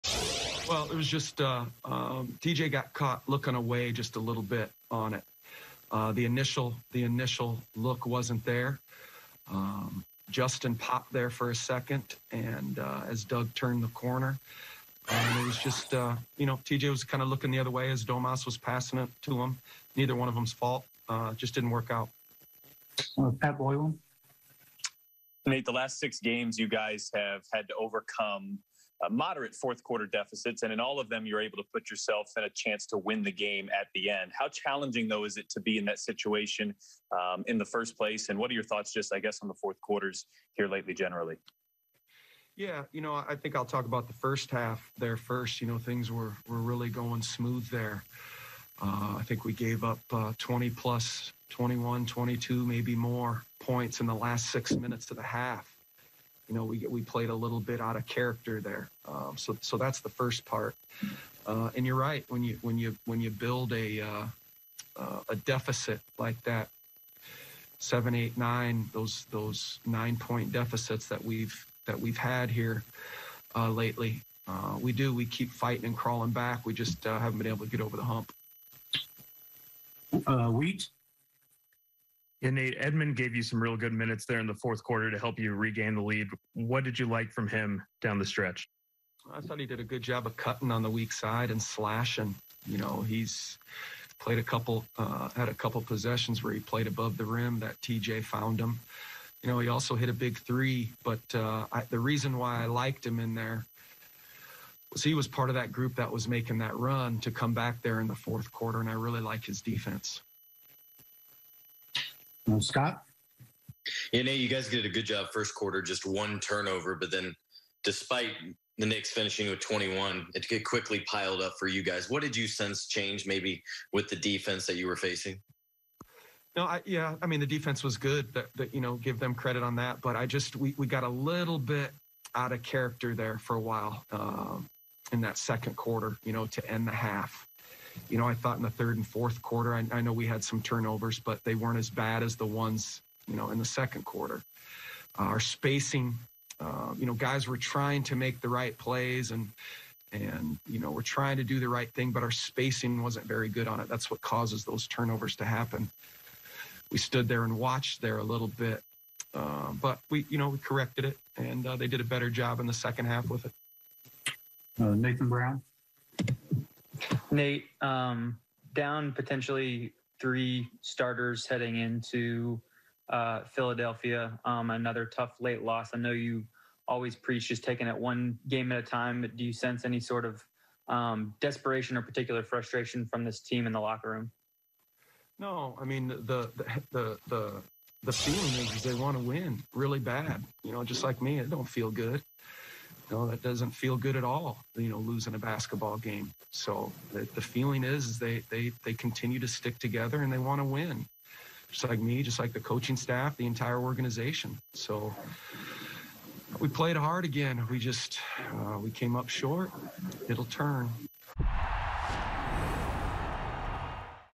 Bjorkgren on final Pacers play: 'Neither one of them's fault, just didn't work out'
Nate Bjorkgren comments on final Pacers play that resulted in a turnover.